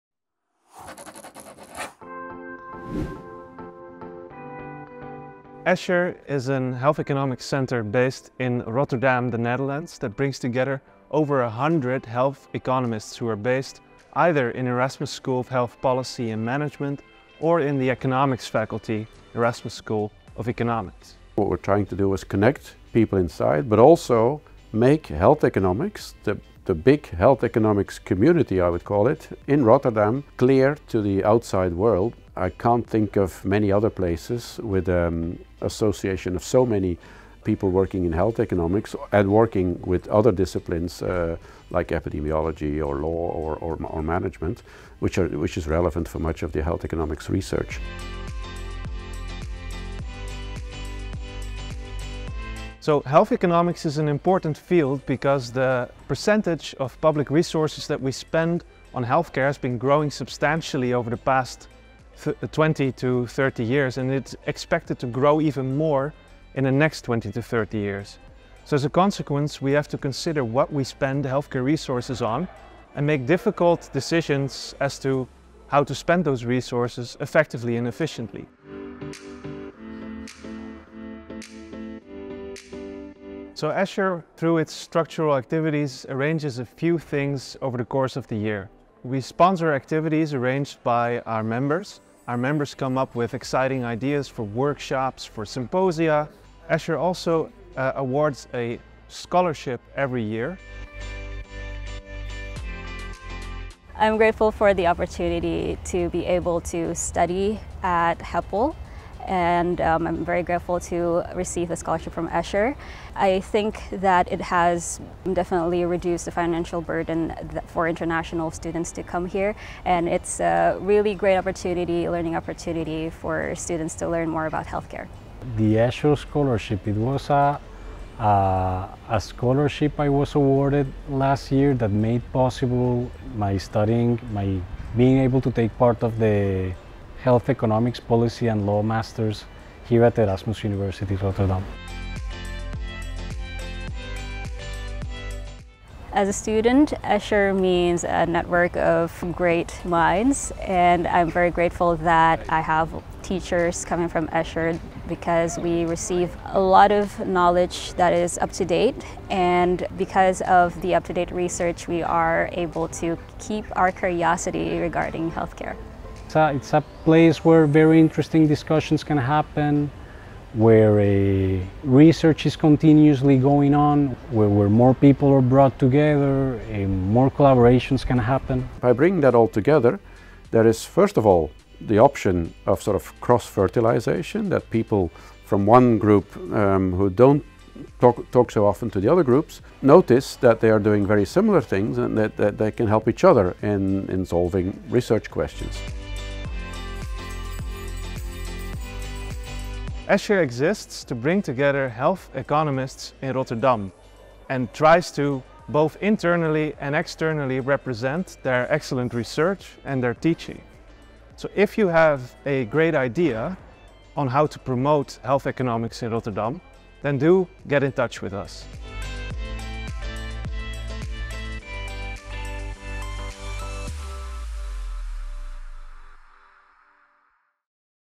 EsCHER: the Erasmus Centre for Health Economics Rotterdam
Watch this video and learn what EsCHER, the big health economics community in Rotterdam, is all about.